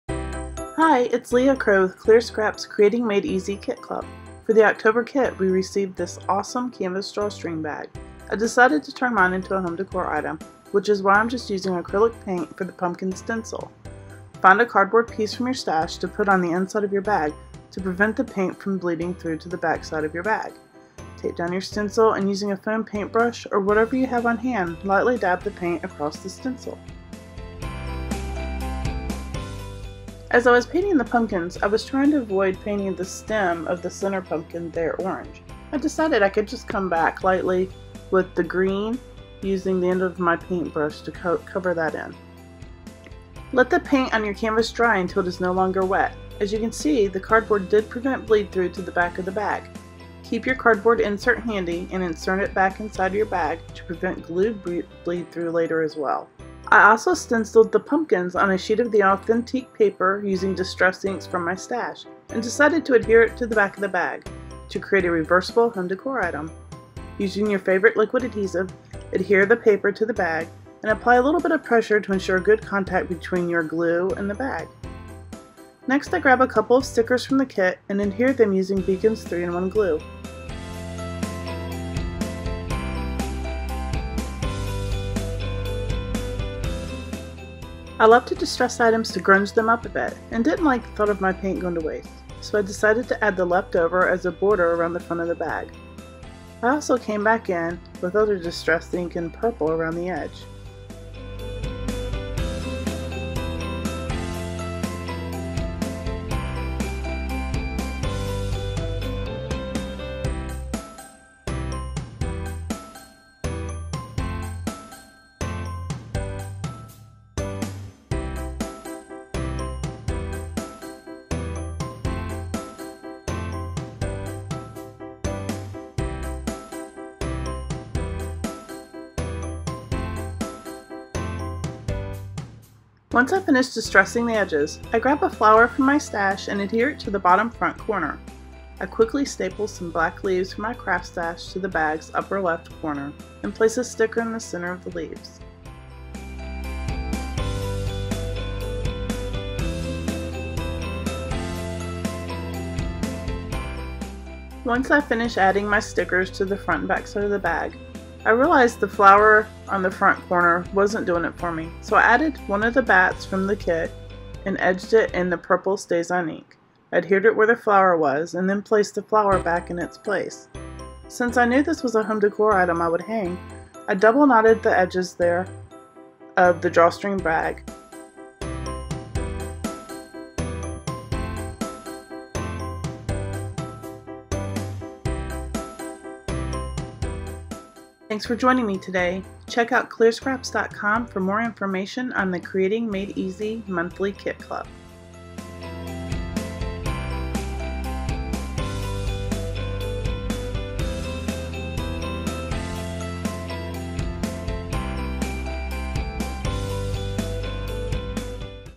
Clear Scraps Creating Made Easy Oct. 2017 kit - Pumpkin Home Decor
Clear Scraps Creating Made Easy October kit, feature the pumpkin stencil, drawstring bag, and acrylic bat.